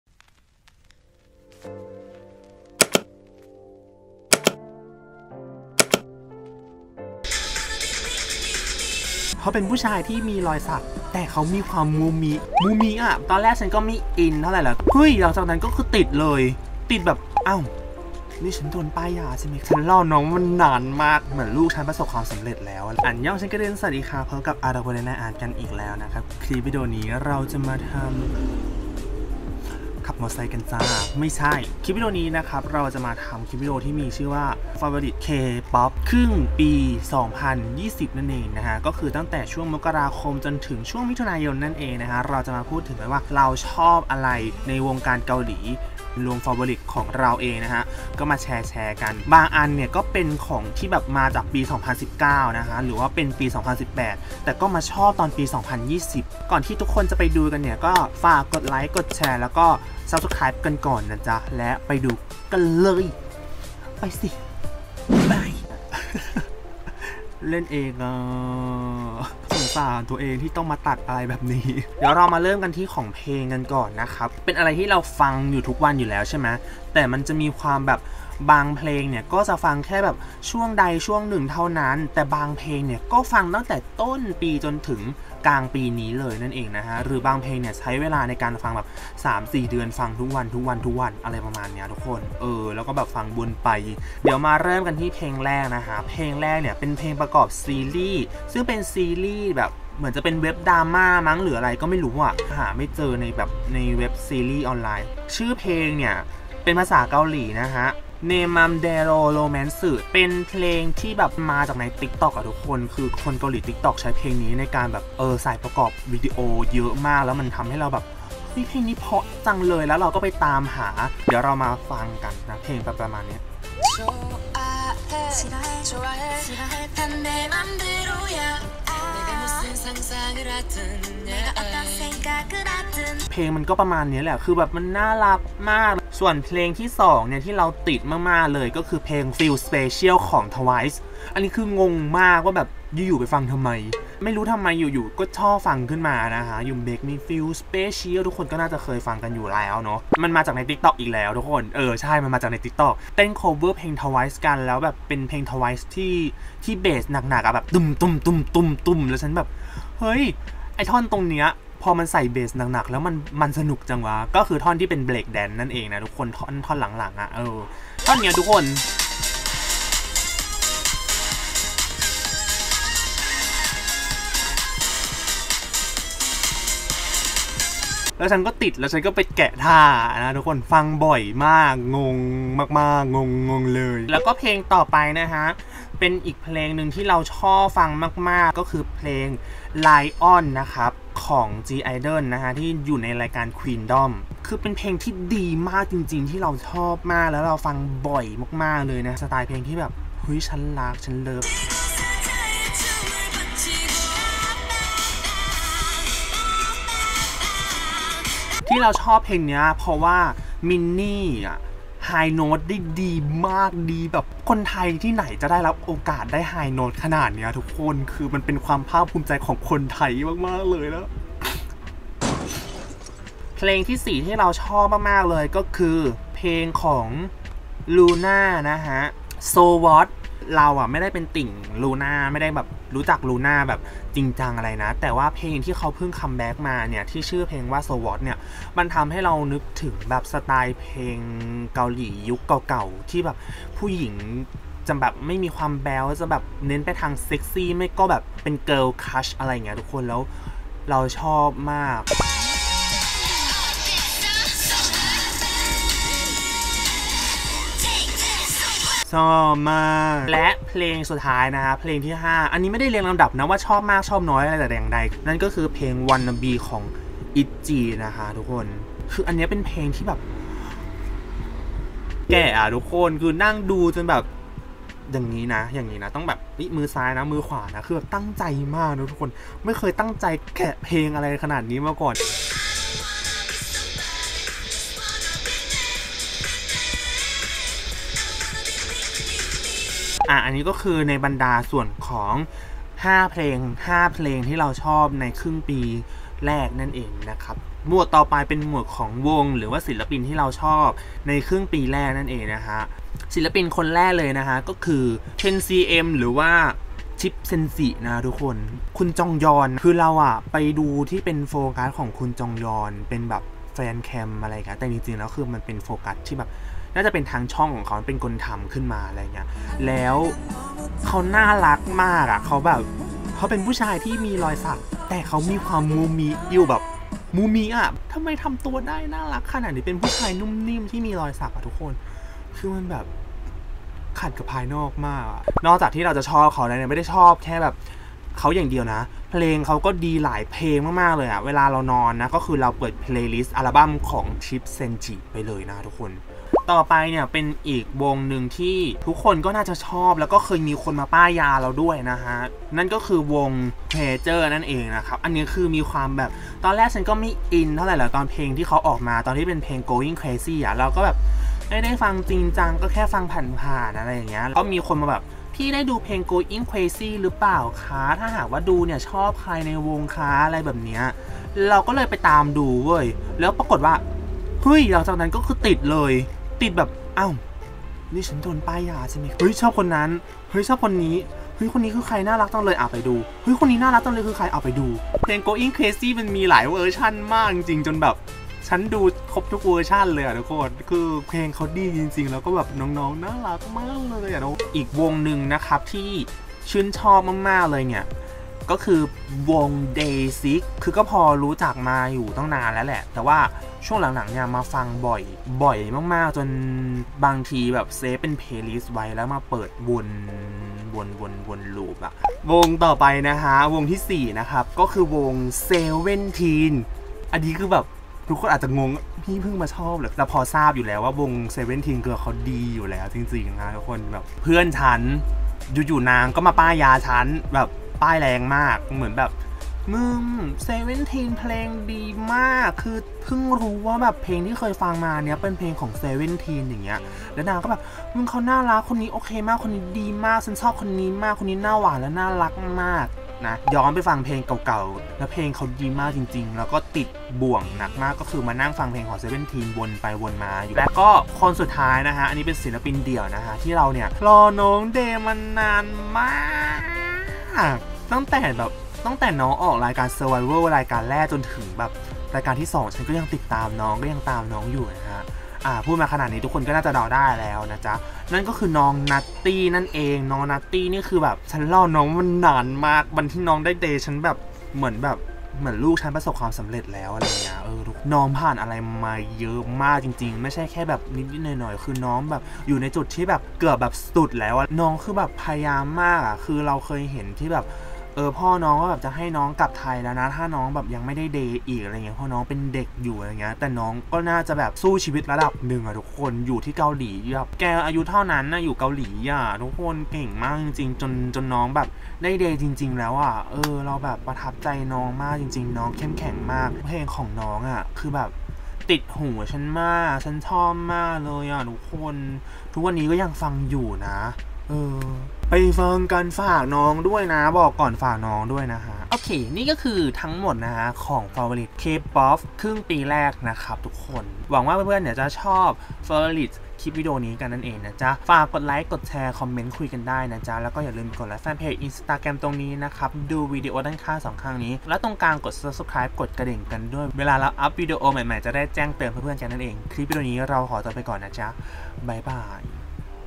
FAVORITE KPOP ♥️ ครึ่งปีแรก เกือบเข้าด้อมอื่นแล้ววว | Arthopotena'Art
อันยองชินกูดึน คลิปนี้มาแชร์ Favorite เกี่ยวกับ KPOP กัน มีวงไหนบ้าง
เพลงอะไรบ้าง มาดูก๊านนนน